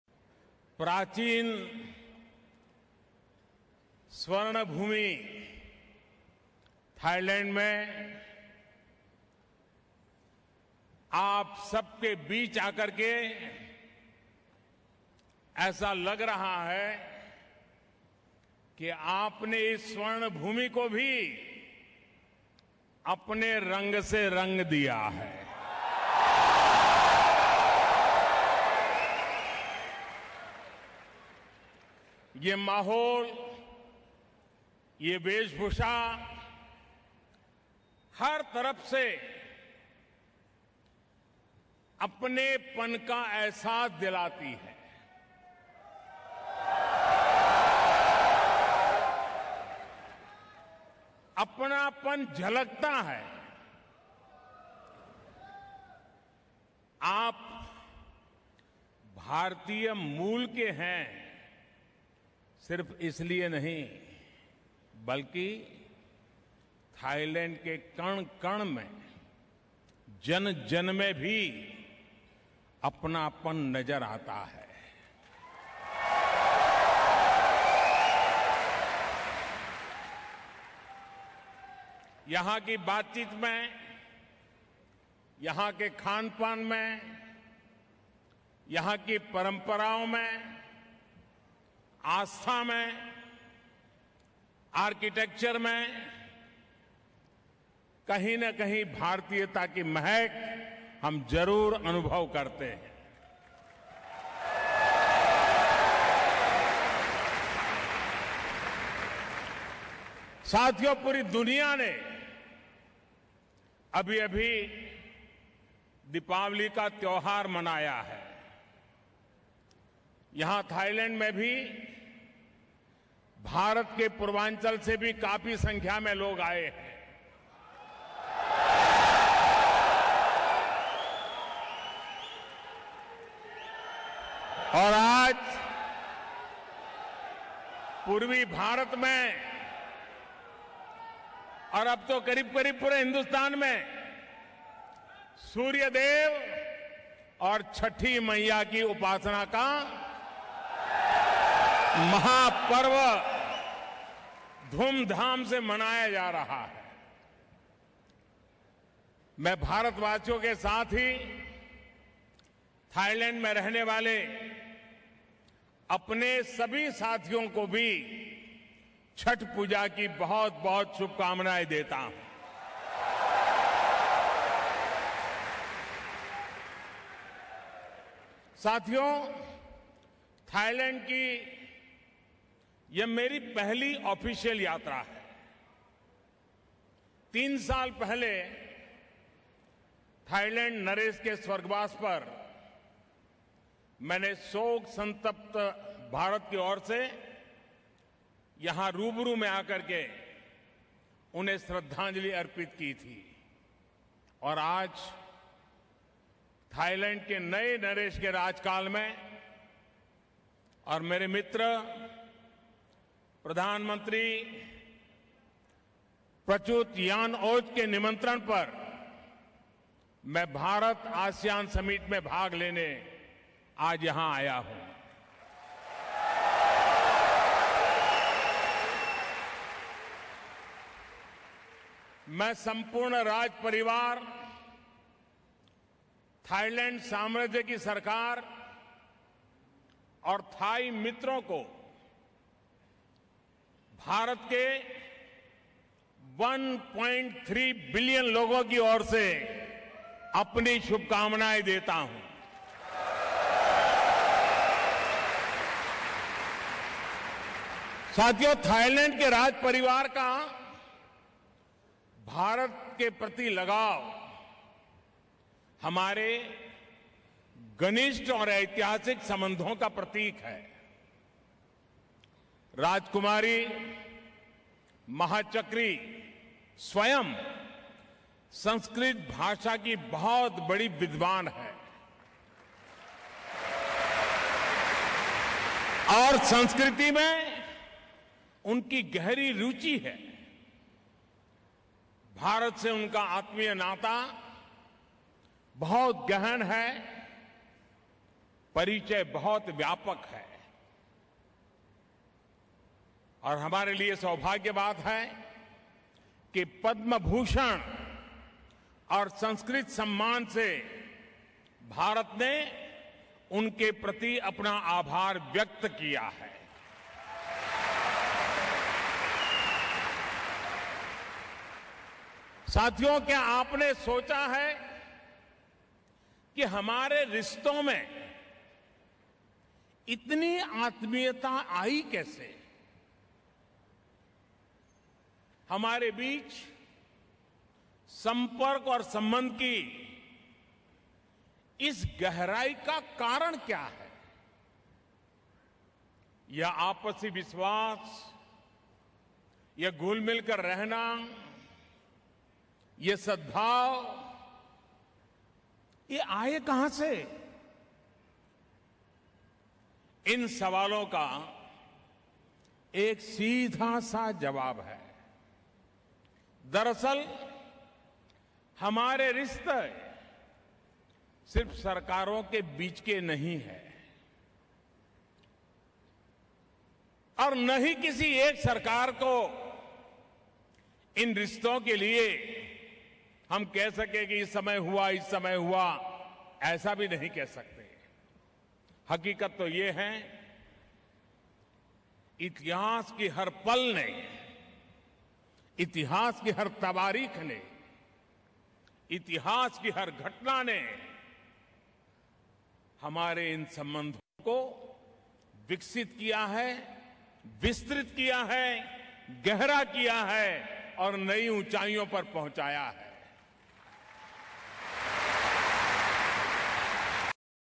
"Ties Between India, Thailand Go Beyond Governments": PM Modi In Bangkok
Prime Minister Narendra Modi celebrated the universality of Indian practices on his first day of his Thailand visit today, claiming that its values and architecture are felt in countries across the world.